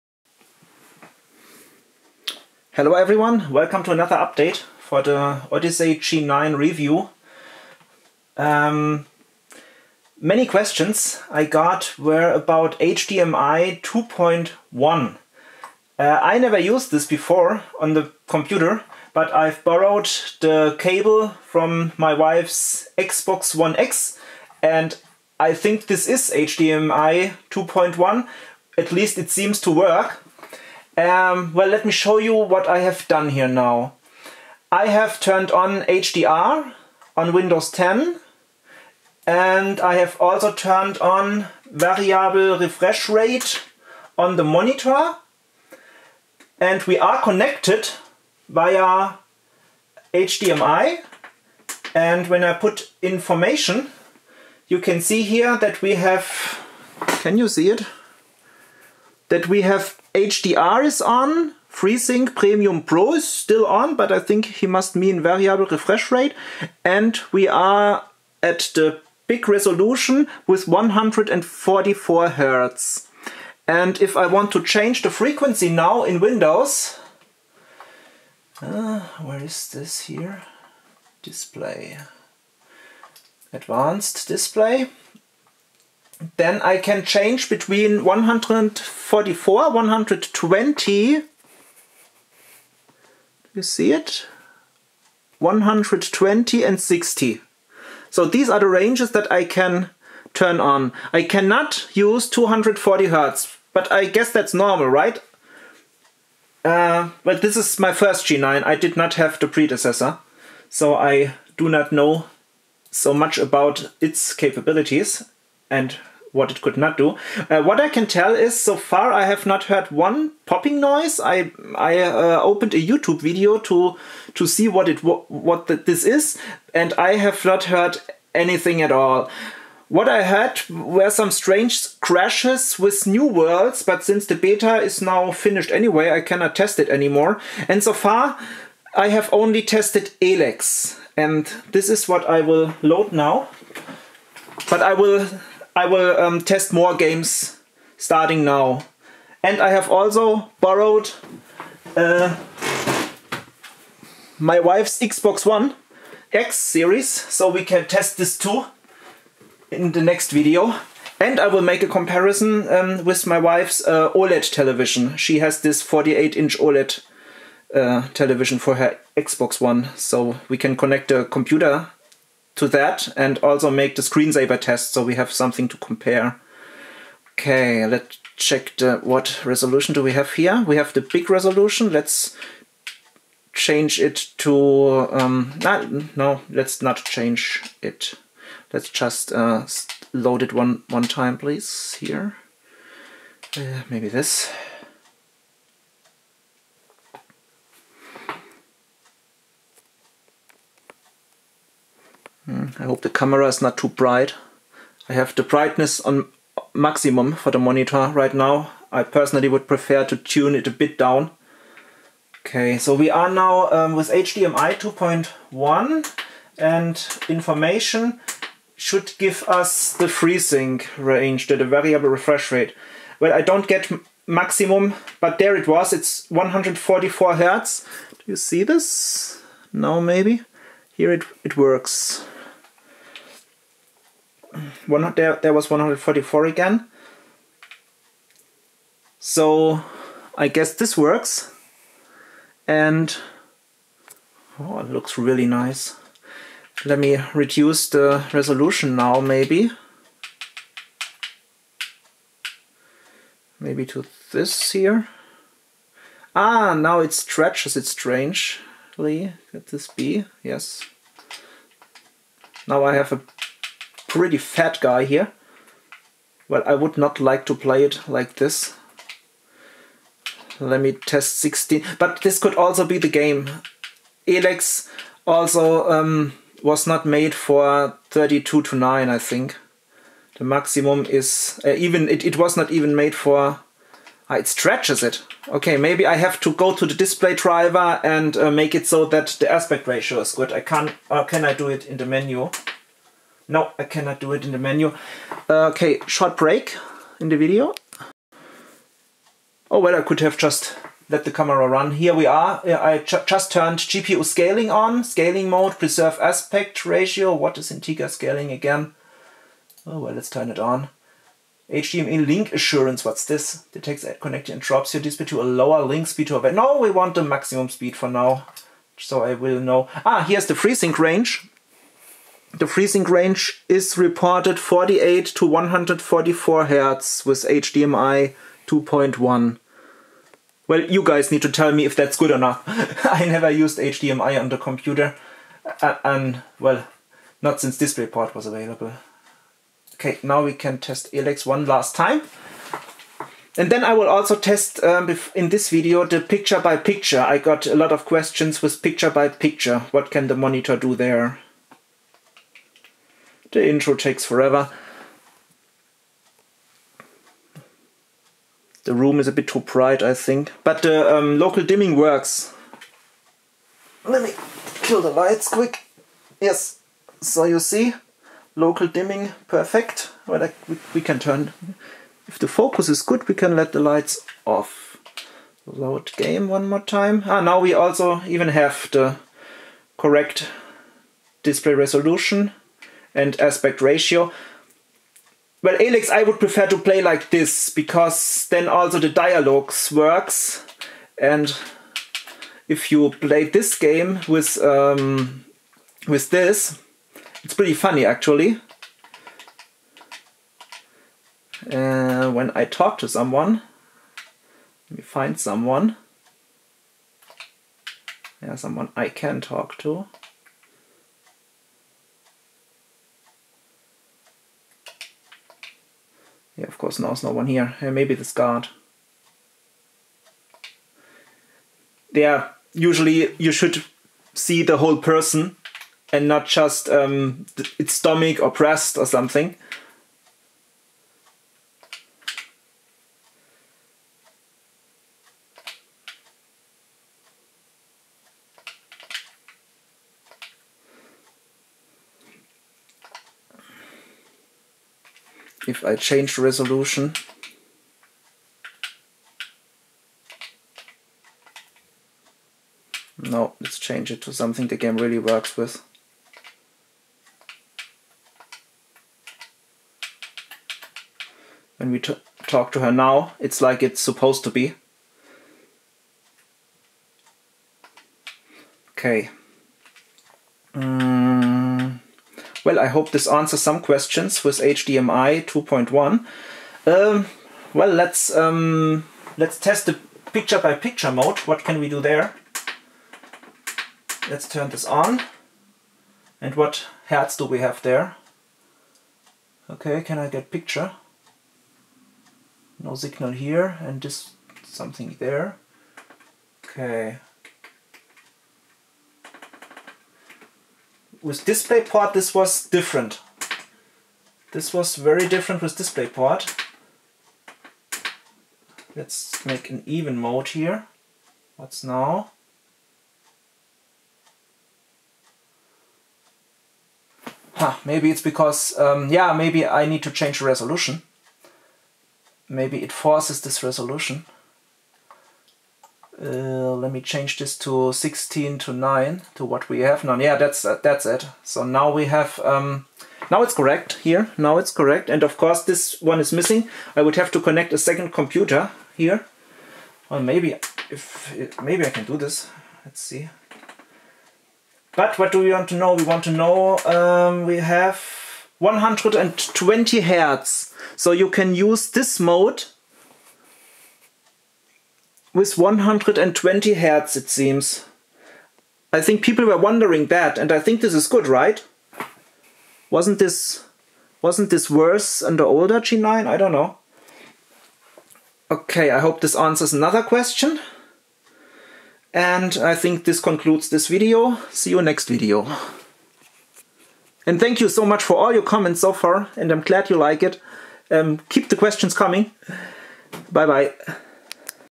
Samsung Odyssey NEO G9 Mini LED HDMI 2.1 Test with different resolutions in Game and Windows 11
My new High Speed Cable arrived and I did the tests again and the results were the same so I guess we can safely say that this Monitor does maximum 144Hz with HDMI and NOT 240Hz :)
ALSO tested is PBP Mode with HDMI2.1!
Welcome to the next part of my small review series about this Monitor. Many questions were asked about HDMI 2.1 capability and I try to answer some here and show what this Piece of Hardware can do.

As this is my first G9 feel free to tell me if I should try anything else or if this is what you wanted to know about it :)